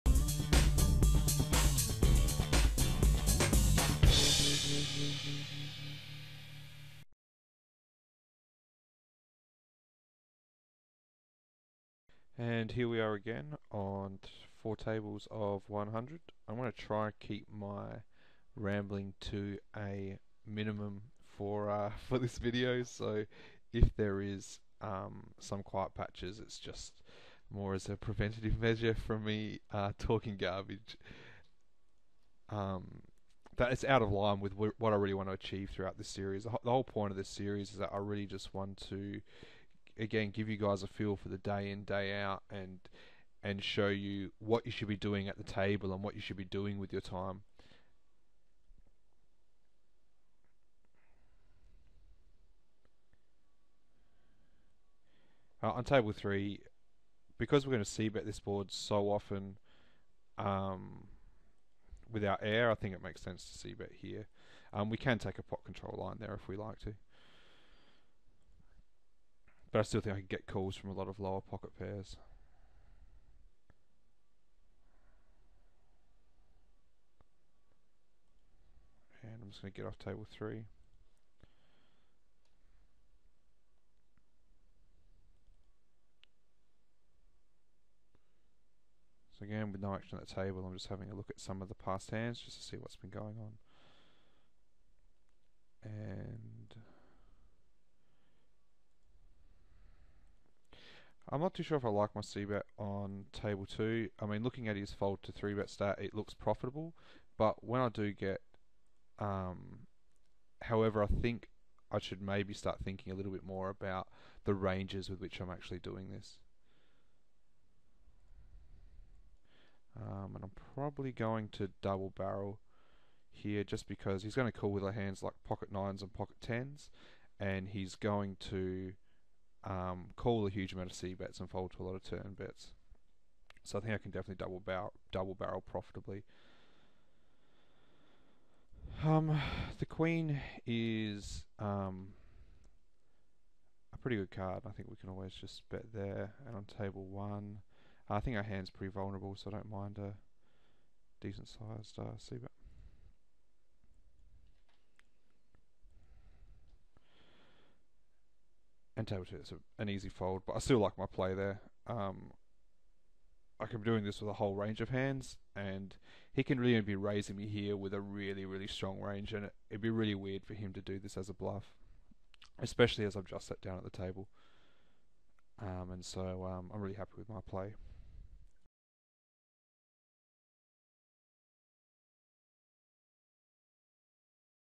Poker Strategy: Josh "TRD23" Pix #6: Hustler's Ambition - Part 3
In Part 3 - TRD23 provides you with a 'Session Strategy' guide, which details what you should be looking to do during your daily grind. Josh then fires up 4 tables of $100 No Limit, and provides live commentary.